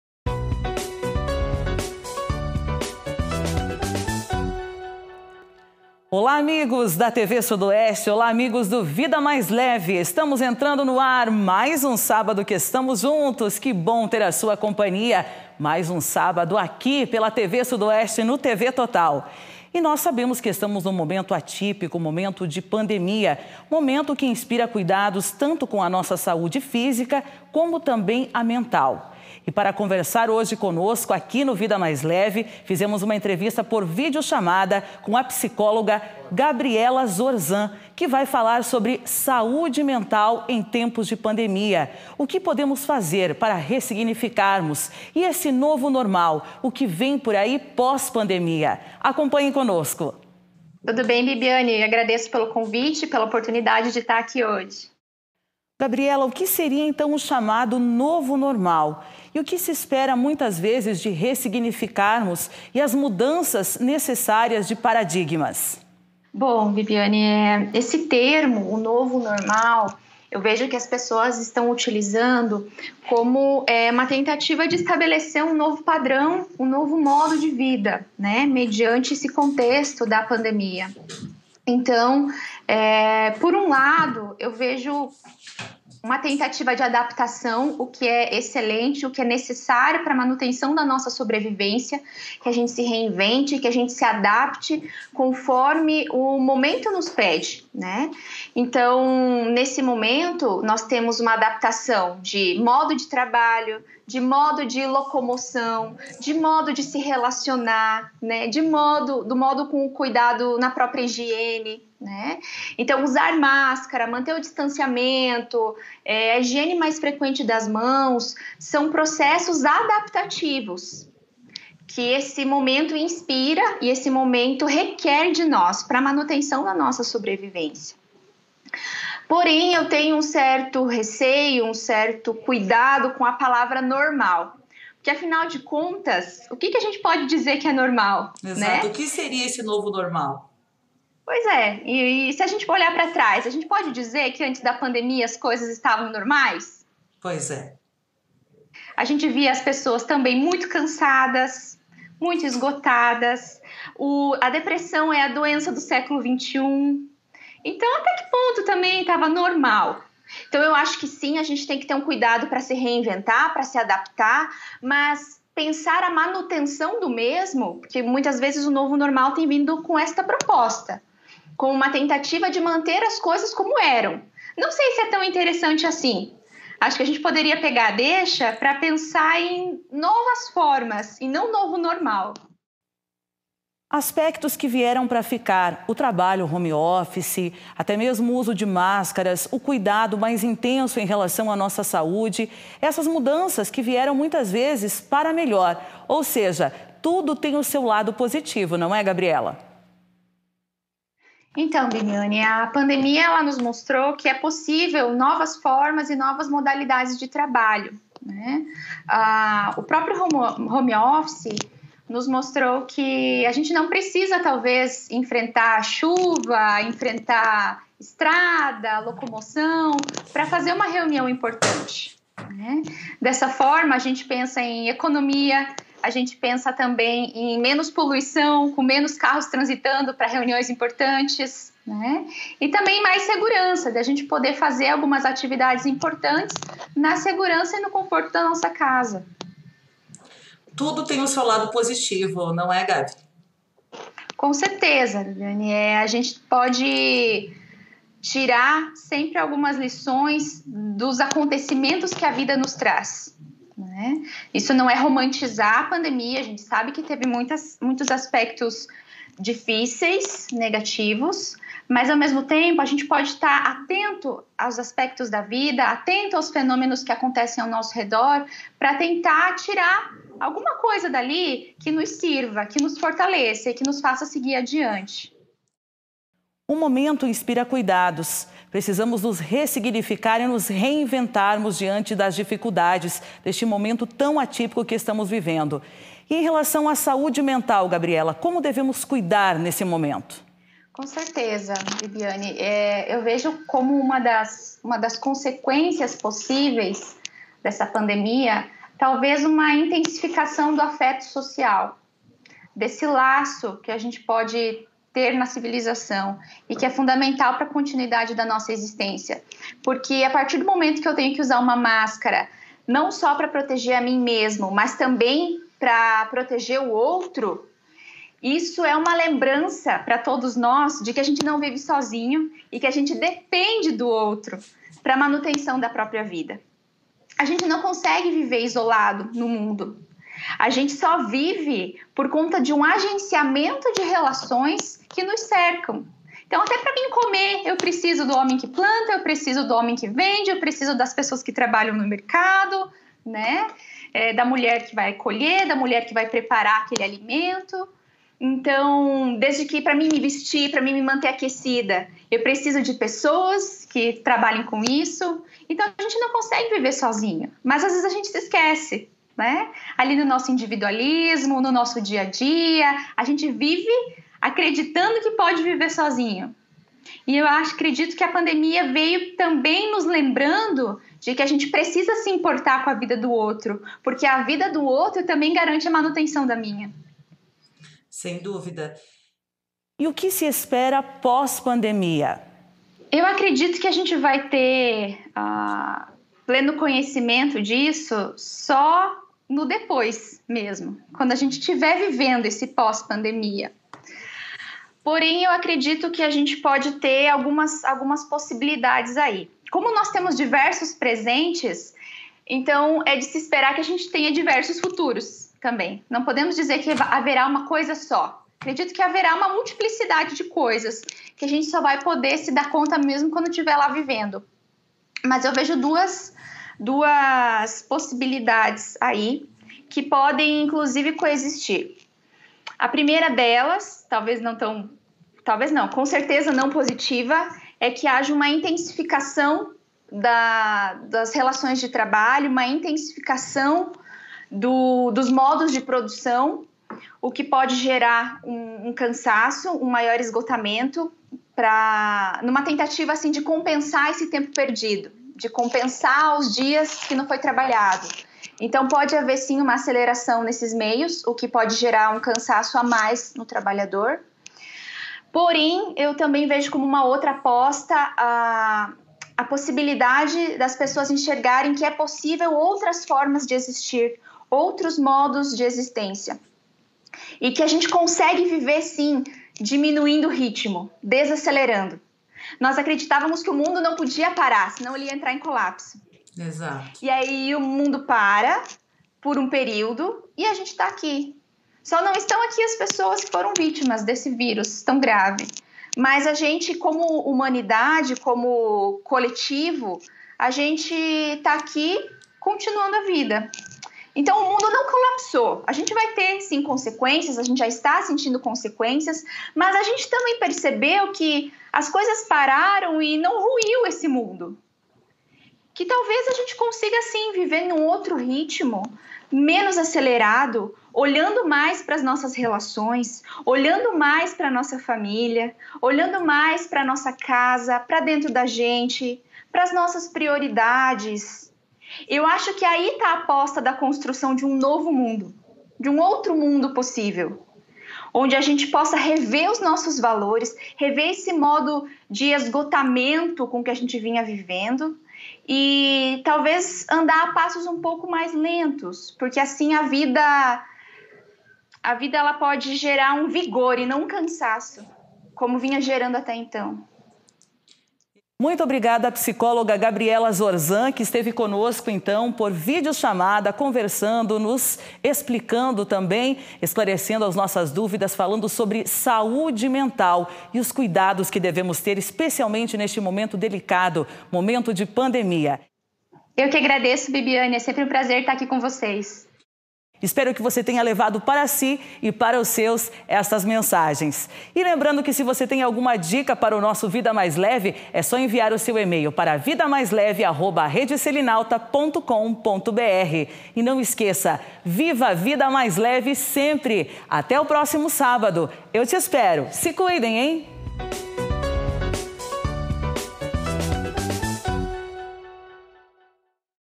Vida + Leve com a Psicóloga Gabriela Zorzan
Saúde mental em tempos de pandemia- como nos reinventarmos e buscarmos o entendimento para cada situação, neste momento atípico e de incertezas.